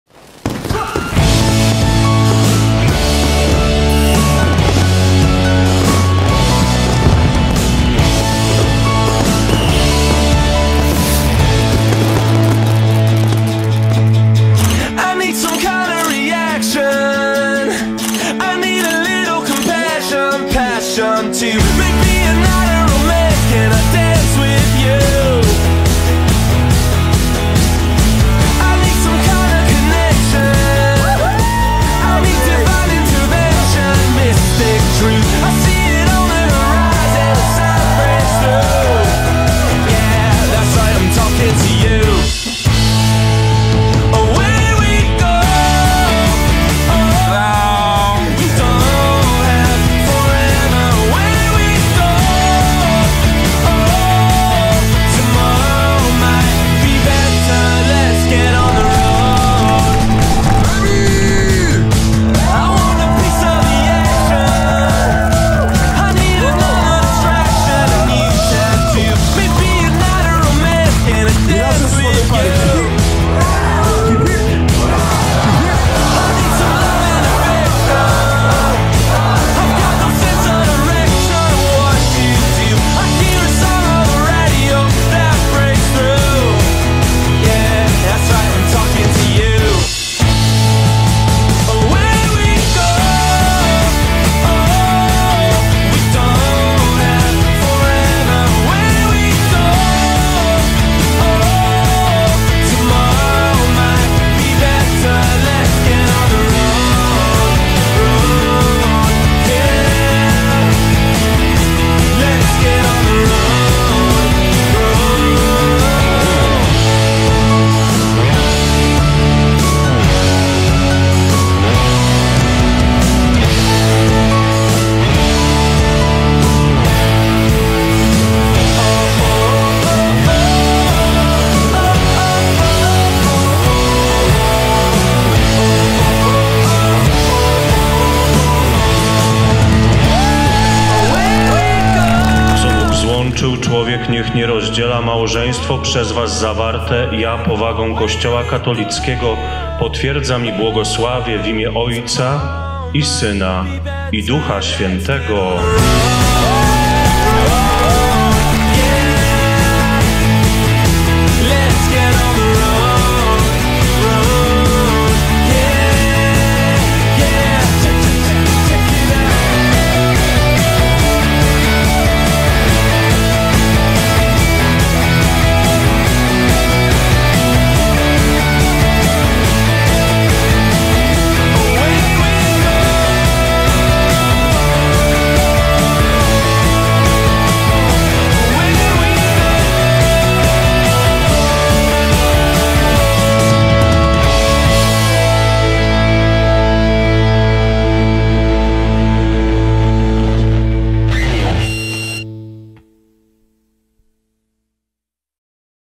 Najlepszy teledysk ślubny 2023!! ❤️😍🔥
Czyli Anna & Emil w swoim żywiole!

🥳 Miejsce: Restauracja Platinum Budzyń
📸 Fotograf: Ślub Się Kręci - Krzysztof Budych Studio
🎧 DJ: Paweł Kucharski - Najdroższy Manager Twojej imprezy! 🔥

Chciałbyś abyśmy uwiecznili Wasz ślub i wesele? Napisz do nas!